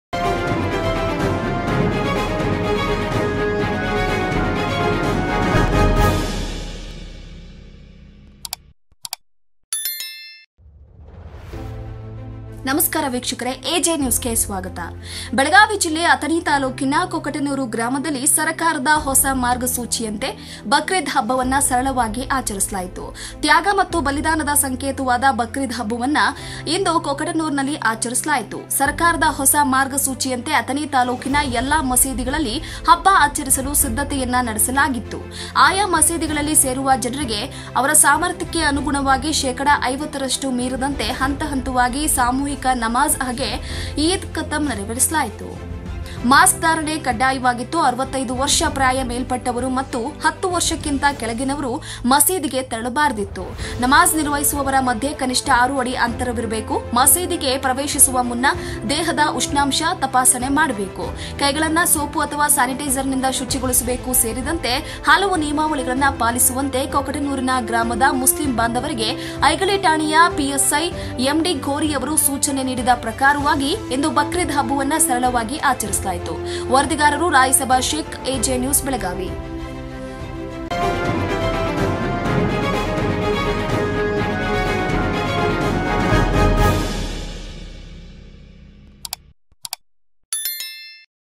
The Bakrid festival was simply celebrated in the Kokatnur village Athani | AJ NEWS BELGAVI
ajnewsbelagavi ajnewsbelagavi ajnewsbelagavi ajnewsbelagav athaninews chikkodinews raybagnews Hubbalinews Hukkerinews bagalkotnews bengaluru news